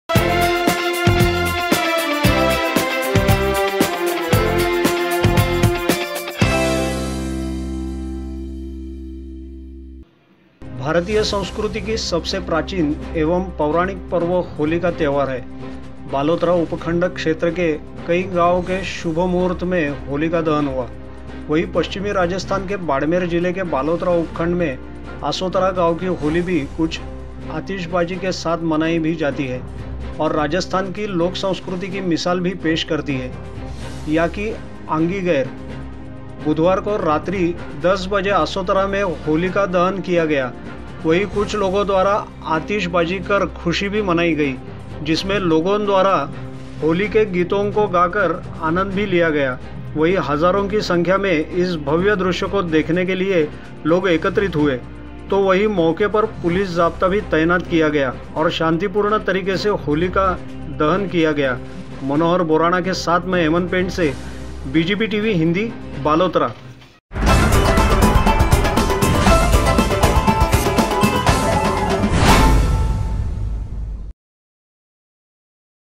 उपखंड क्षेत्र के कई गांवों में शुभ मुहूर्त में हुआ होलिका दहन आसोतरा में होली दहन पर उमड़ी भीड़
Reporter            :  Manohar Borana

voice Over         Hemant Pendse

Editor                 : Hemant Pendse

अभी आप बनो आपके  विभाग के पत्रकार

हम दिखाएंगे आपकी खबर अपने BGB Tv पर

अब लढ़ो अपने हक़ के लिये